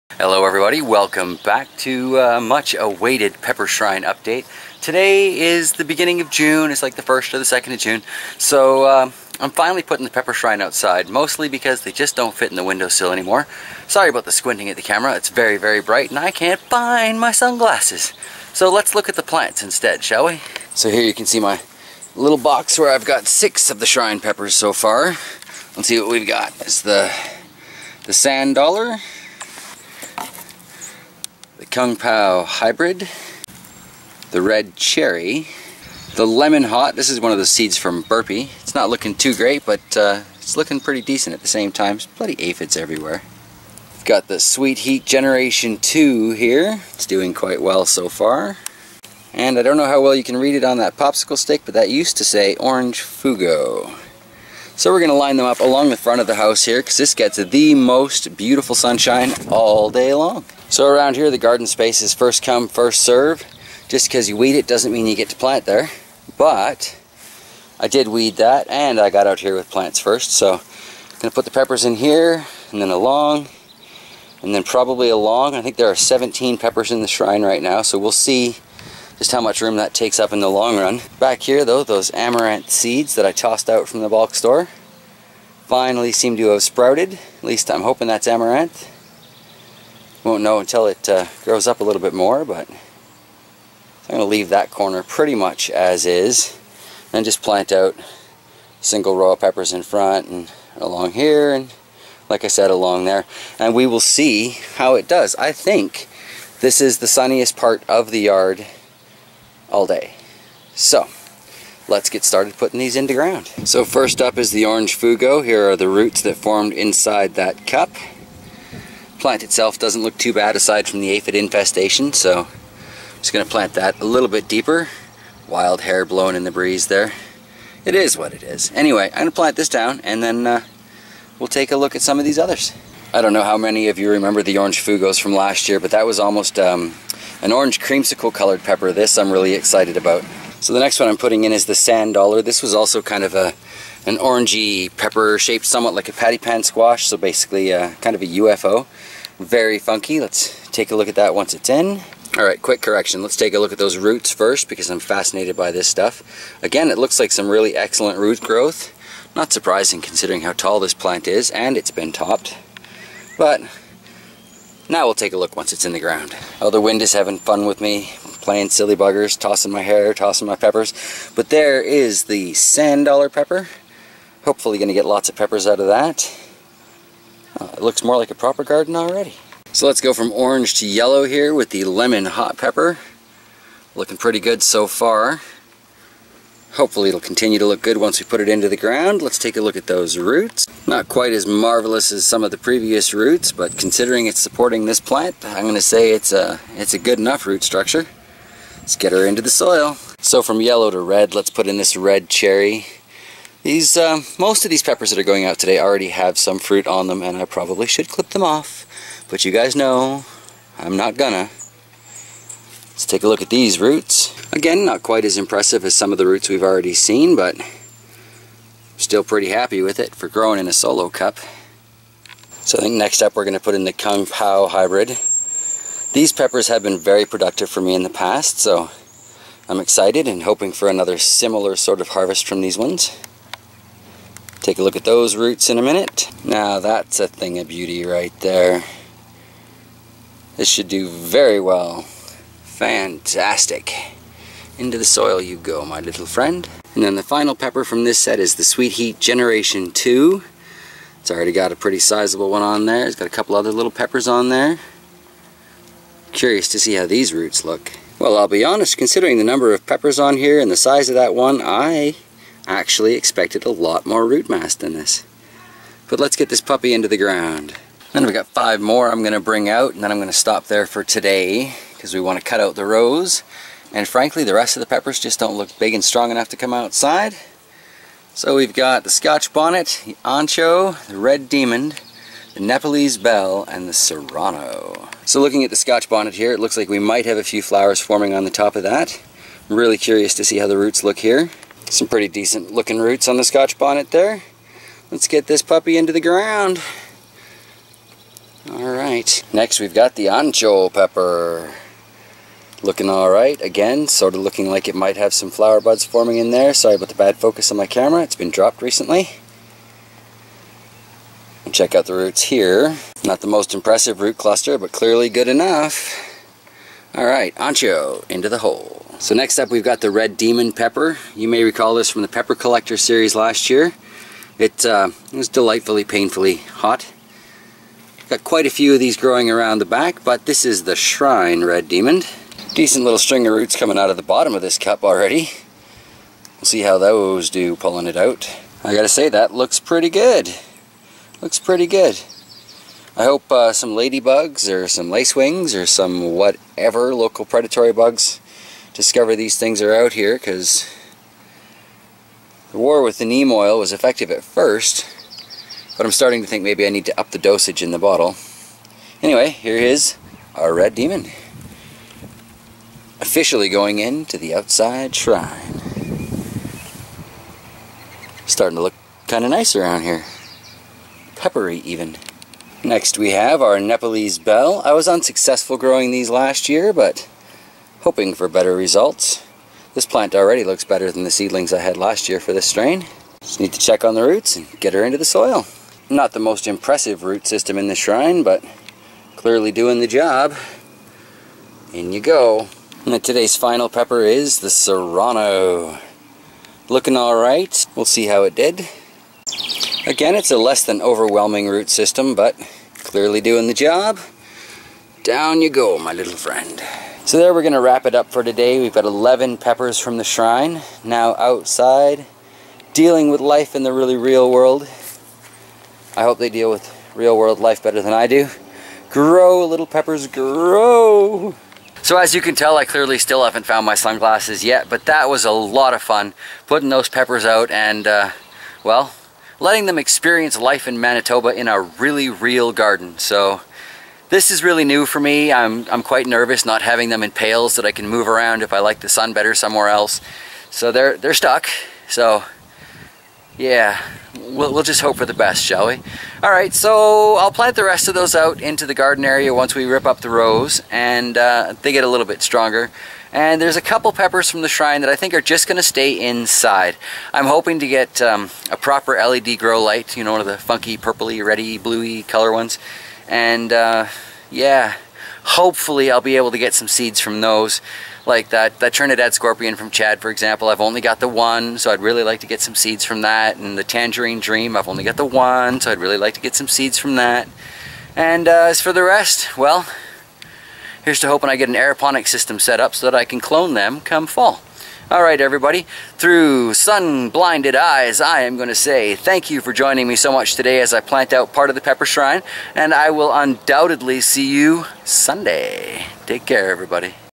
Pepper Shrine 2016 Planting the 11 Outside
Hey there everybody, welcome to the long awaited Pepper Shrine update!  These have been doing fairly well but they've gotten a bit too big for our little window sills, so it time for them to go outside!
Out of the 17 different pepper types that are in the shrine at the moment, 11 of them are outside in the soil as of today.  Here's to hope!
Enjoy the collection, have a wonderful day!
Happy harvests & blessed be.

Some of the other channels, if you're interested .